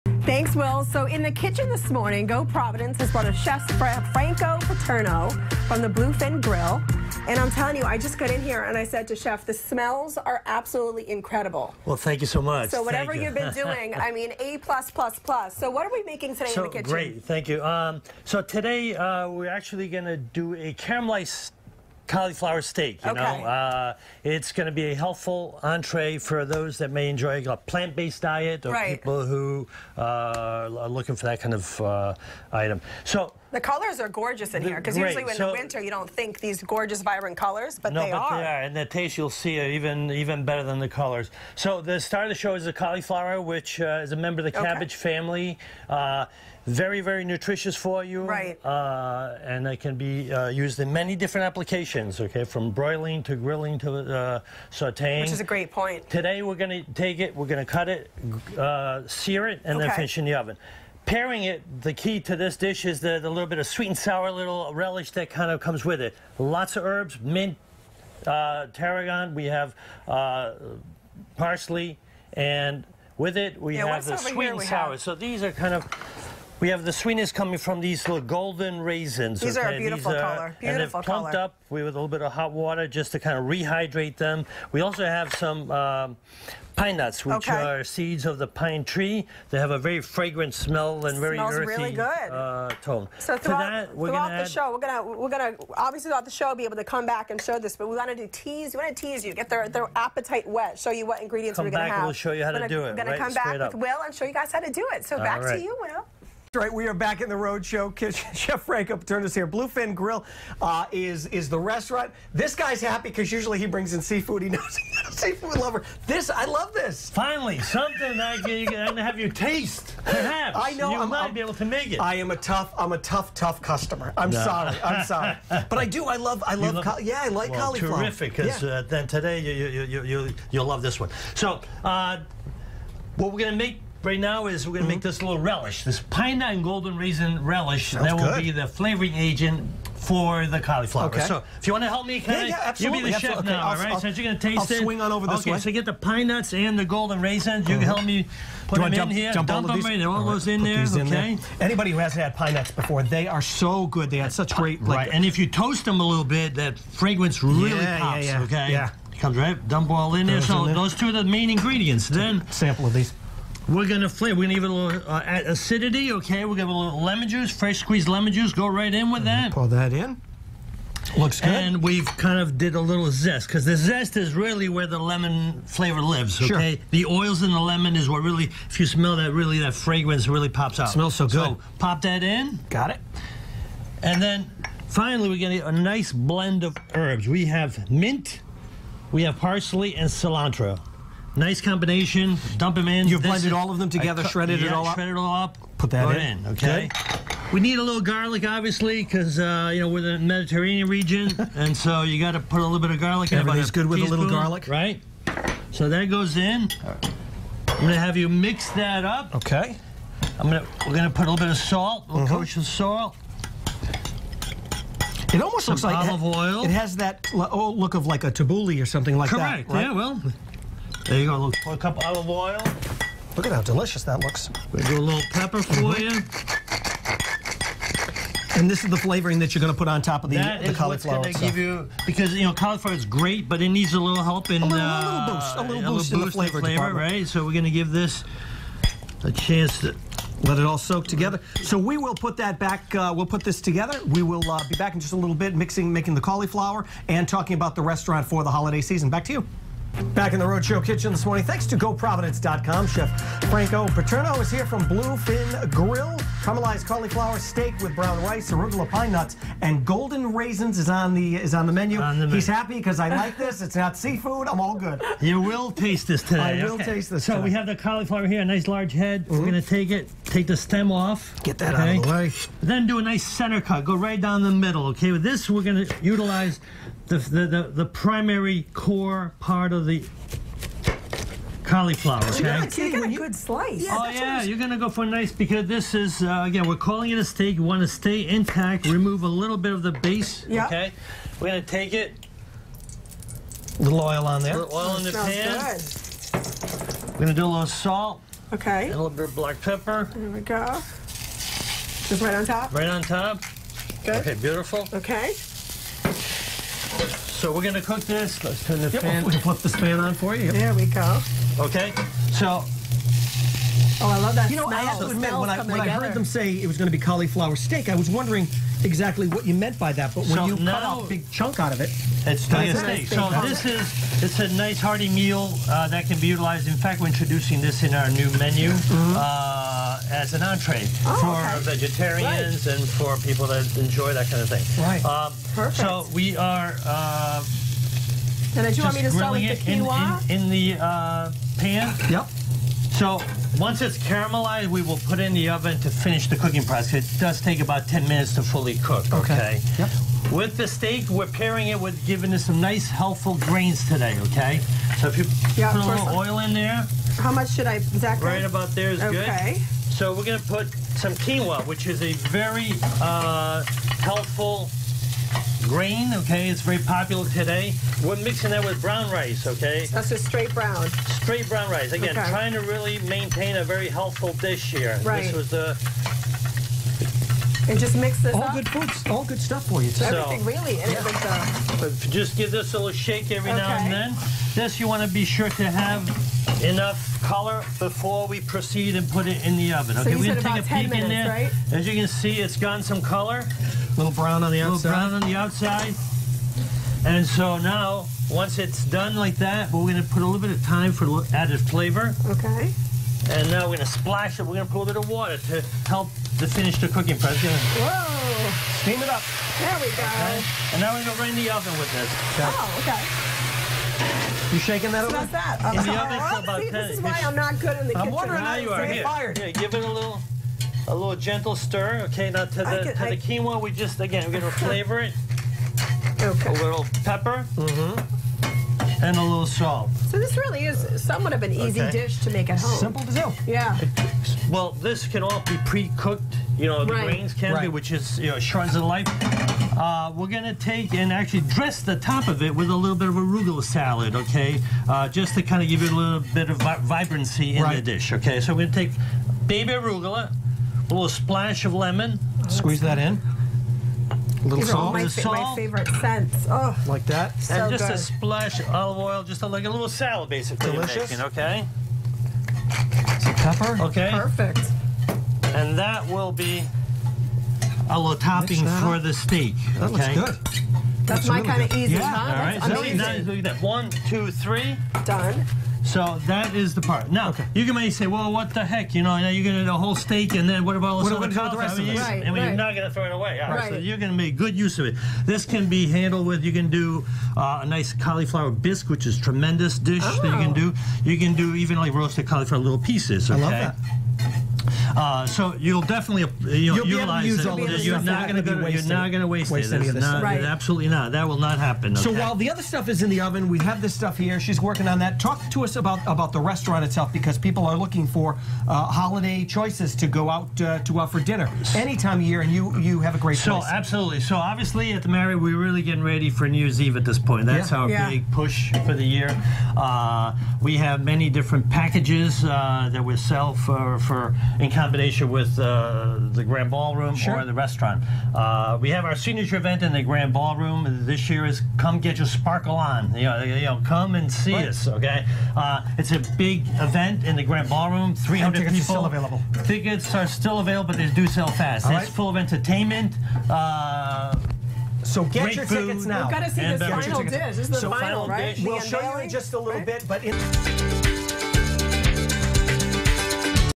In the Kitchen: Caramelized Cauliflower Steak
In the kitchen today was Executive Chef Franco Paterno from Bluefin Grille/Providence Marriott Downtown making Caramelized Cauliflower Steak with Brown Rice, Arugula, Pine Nut, & Gold Raisins